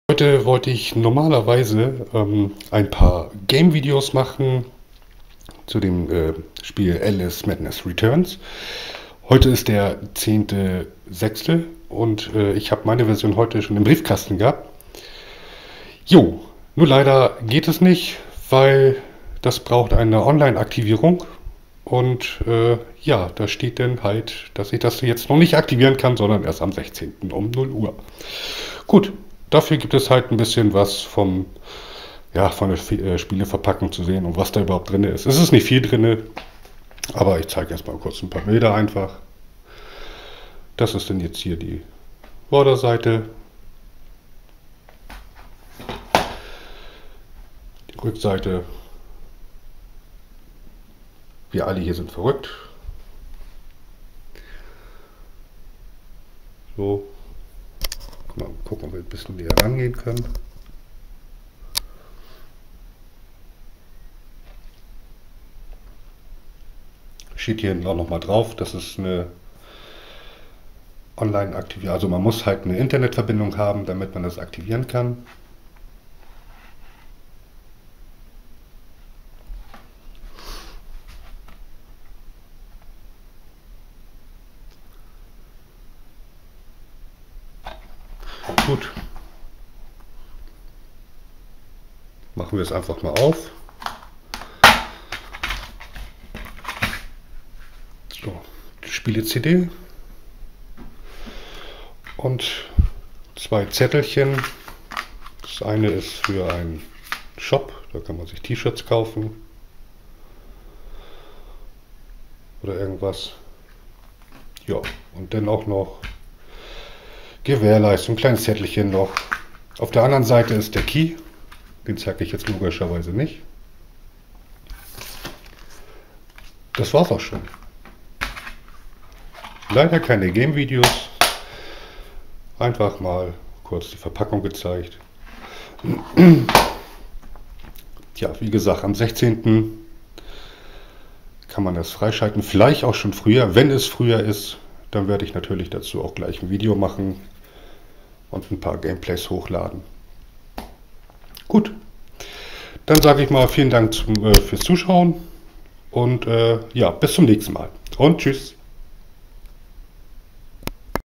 Unboxing: Alice - Madness Returns - PC
YO !! Normal wollte ich ja ein paar Gamevideos machen, als ich am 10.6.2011 meine Version von Alice - Madness Returns im Briefkasten hatte.

Game installiert, Fraps gestartet und dann Gamegestartet - Aber moment !! Release Date Check ? BÄHM - Das offizielle Erscheinungdatum dieser Anwendung ist: Donnerstag 16. Juni 2011 0 Uhr :-(

Najut, dafür gibt es dann halt ein Video von der Verpackung :-) und am 16. Juni gibt es ein Video von den ersten 20 Minuten von Alice Madness Returns :-)